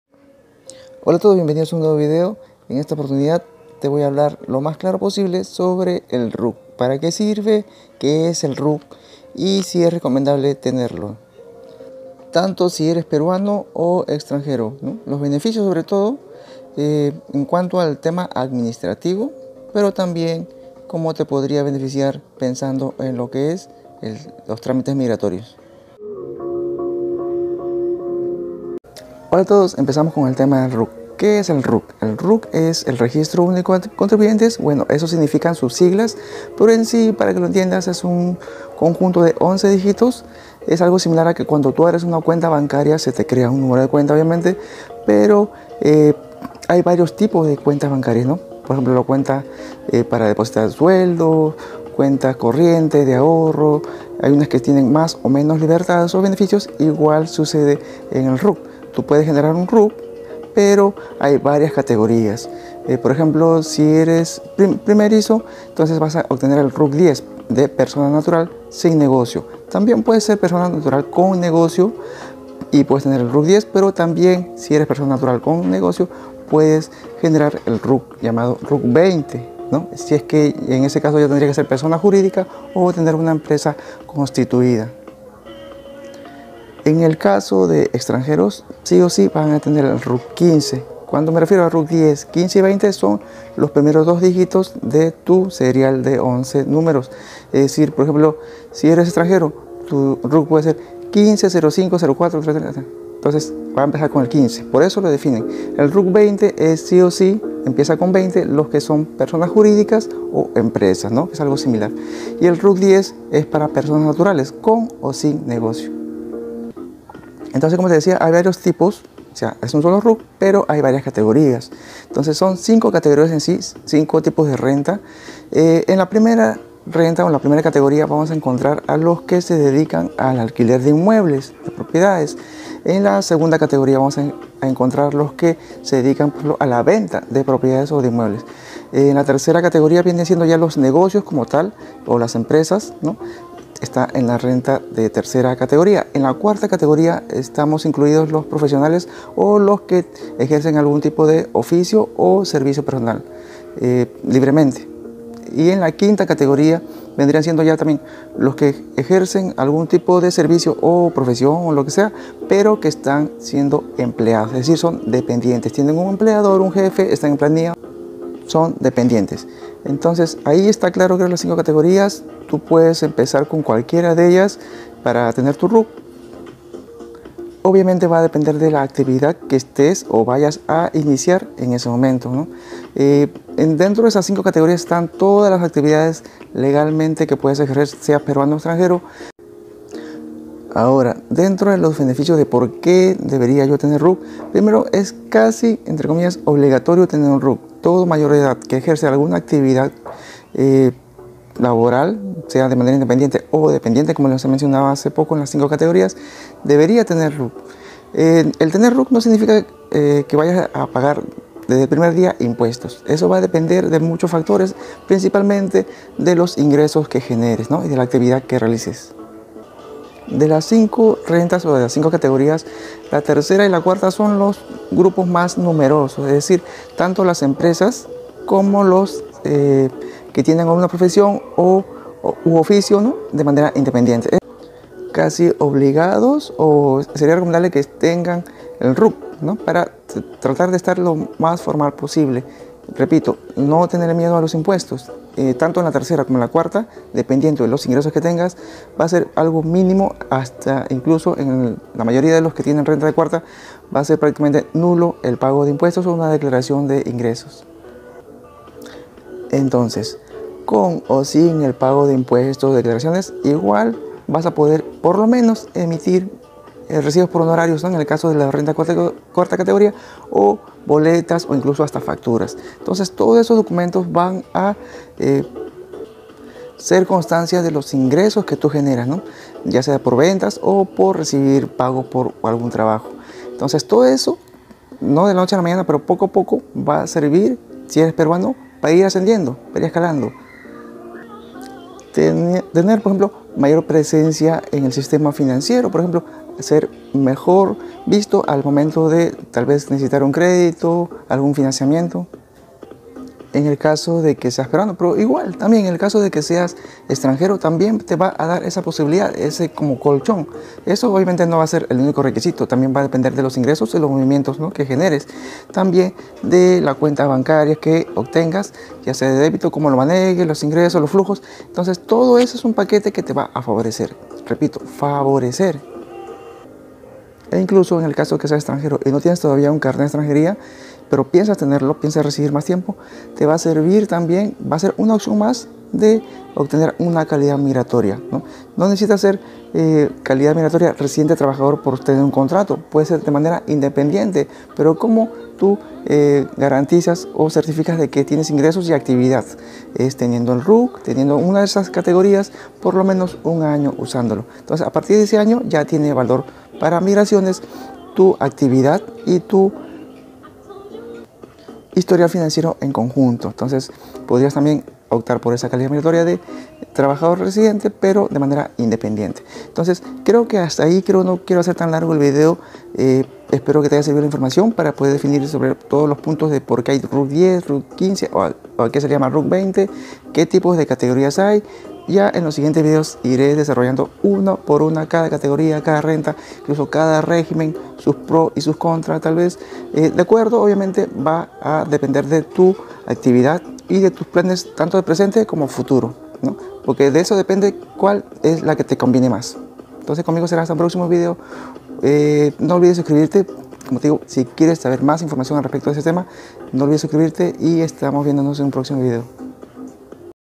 RUC 2023 /Que es el ruc , para que sirve , es para peruanos y extranjeros
QUE ES EL RUC, PARA QUE SIRVE , DEBEN TENERLO PERUANOS Y EXTRANJEROS ?